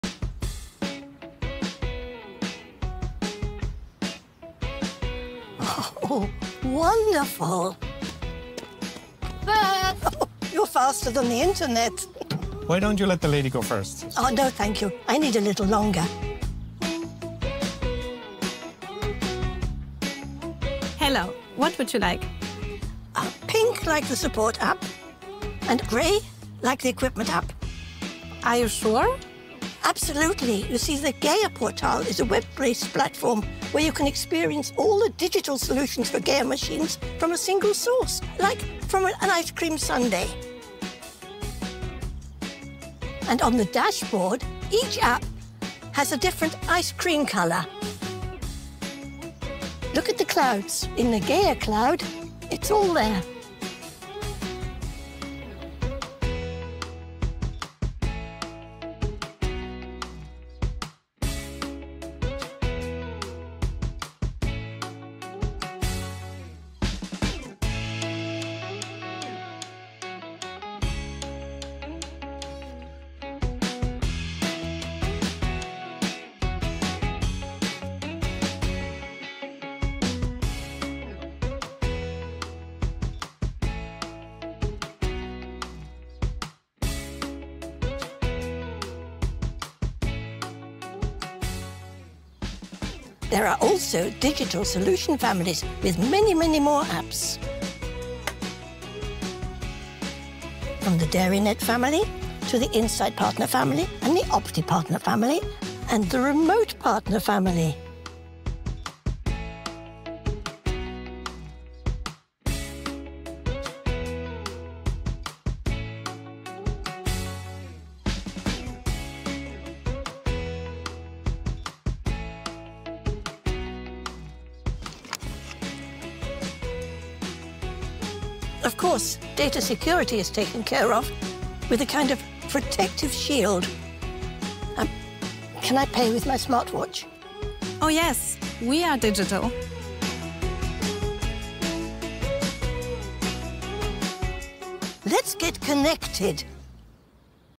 Let’s get connected - all digital solutions in one place
Meet the GEA Portal - your single digital entry point to smart digital solutions. We’ve brought every GEA digital solutions together into one intuitive platform to eliminate complexity and streamline your daily workflow. With instant online access - anytime, anywhere - the GEA Portal gives you a live overview of machine performance, upcoming service activities, open requests, and your complete technical documentation, all in one place. No more searching, no switching between tools - just total clarity and control. This is more than a maintenance platform - it’s your gateway to the Industrial Internet of Things (IIoT). By turning data into real-time intelligence, the GEA Portal helps you boost uptime, plan smarter, reduce costs, and drive sustainable long-term performance across your entire operation.